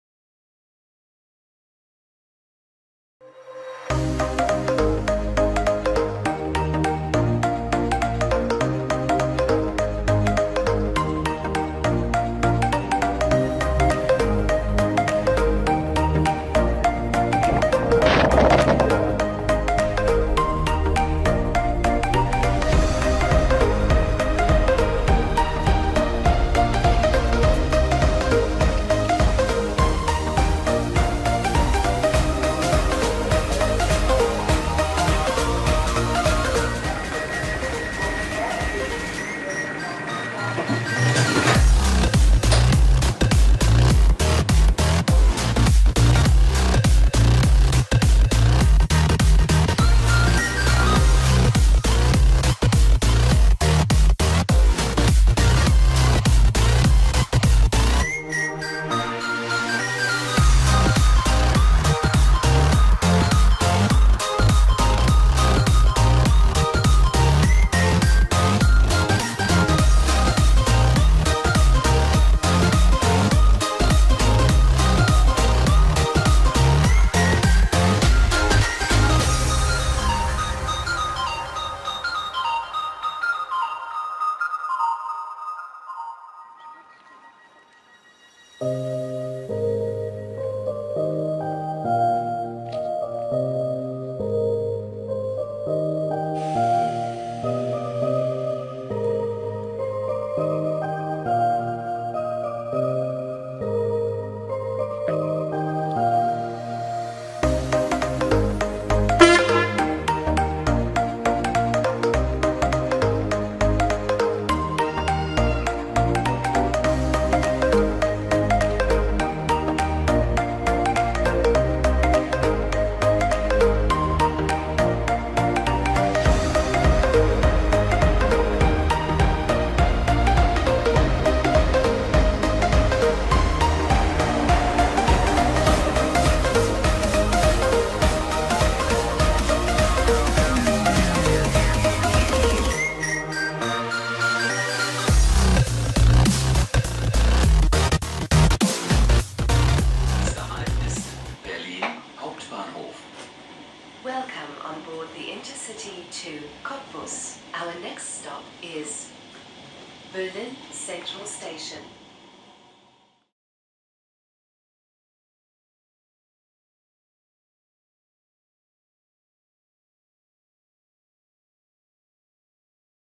IC2 Twindexx das erste mal planmäßig im VBB / 13.12.2015
Bevor ich was zum Video sage, möchte ich mich bei euch für 200 Abos bedenke !!! Ohne euch wäre ich nie soweit gekommen !!!

Als kleines extra, ein Video von meiner ersten Fahrt mit dem IC2 (Twindexx) in Berlin-Brandenburg, von Potsdam über Berlin Wannsee, Berlin Hbf, Berlin Ostbahnhof nach Königs Wusterhausen. Der IC verkehrt  als IC 2431 der IC linie 56 von Emden Hbf über Oldenburg - Bremen - Hannover - Braunschweig - Magdeburg - Berlin nach Cottbus (mit Ausnahme am Samstag, nur bis Berlin Ostbahnhof ), dasselbe von Cottbus nach Norddeich Mole als IC 2432.
Bei Fragen einfach ein Kommentar schreiben.
Viel Spaß beim Anschauen,
Technik ©